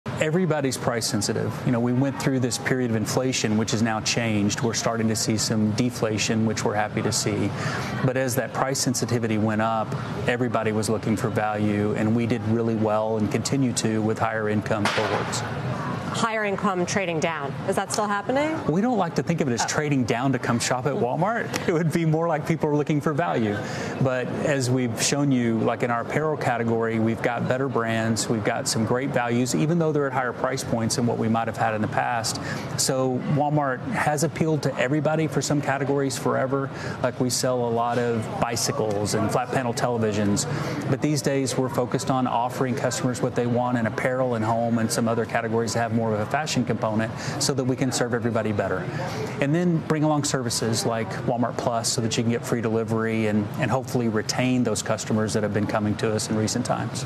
Walmart CEO: Customers are really price-sensitive right now
In a CNBC interview, Walmart CEO Doug McMillon said consumer spending is tougher to predict next year because of rising credit card balances and dwindling household bank accounts.